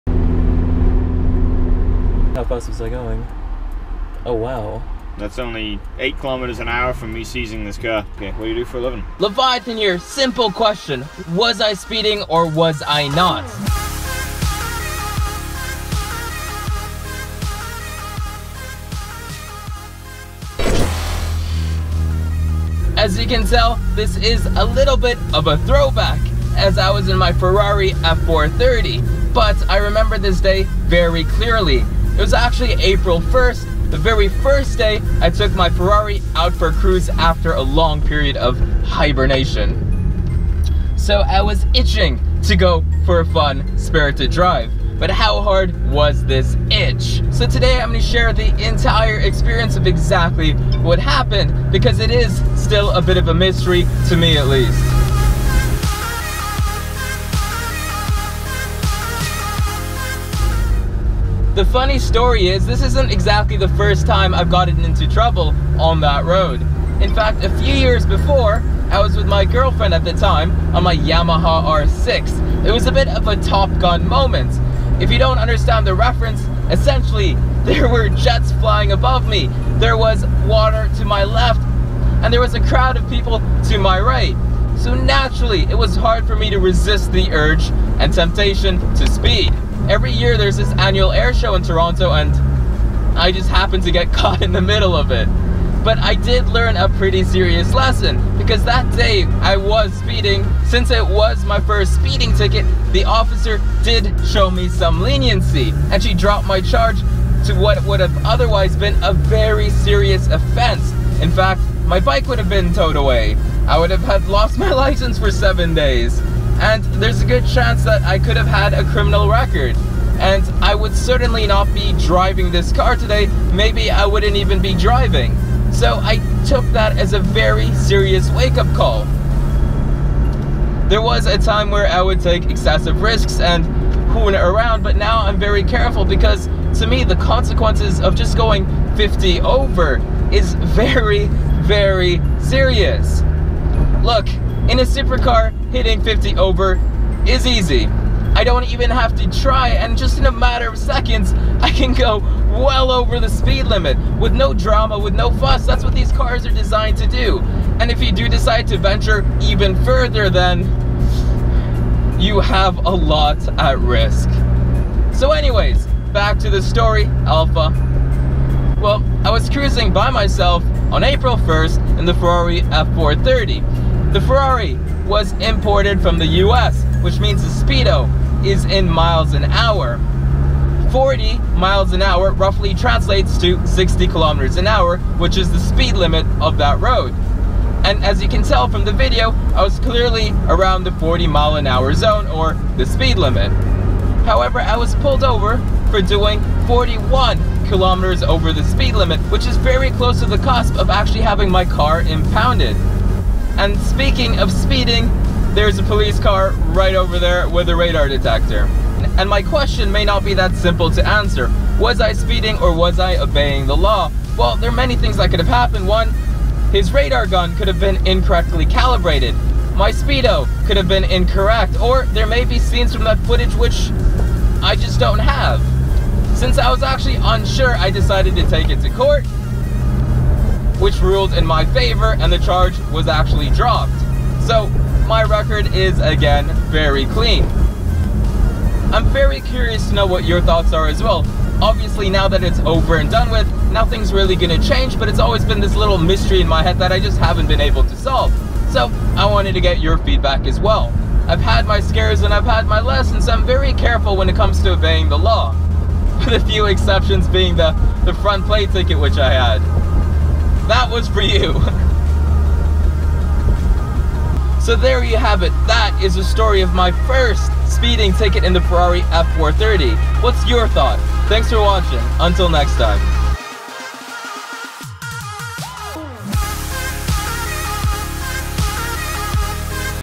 Ferrari Vs Police - ALMOST IMPOUNDED MY F430!
A bit of a throwback, but a few years ago I was pulled over in my Ferrari F430 for going 42 KM over the speed limit, which is very close to being towed and having my license suspended for 7 days. The question I have for you is, was I speeding?